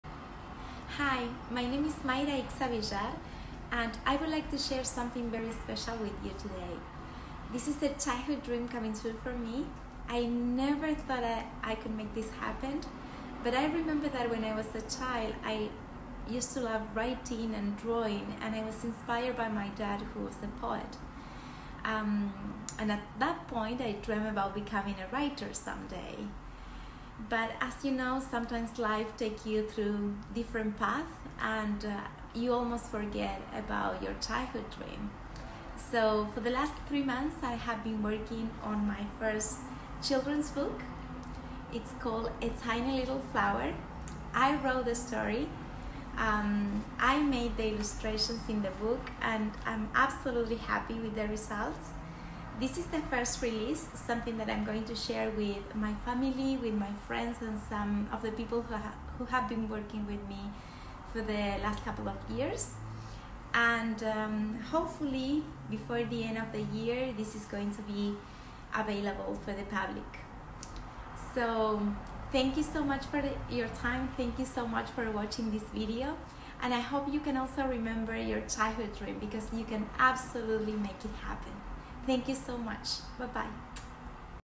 My first children's book!!!
I can’t express with words how happy I am with this announcement: my first children’s book!!! 😊 This used to be a dream, just a fantasy, I never thought I could actually hold it in my hands one day but my favorite author and book couldn’t say it better: “...when you want something, all the universe conspires in helping you to achieve it.” - Paulo Coelho, The Alchemist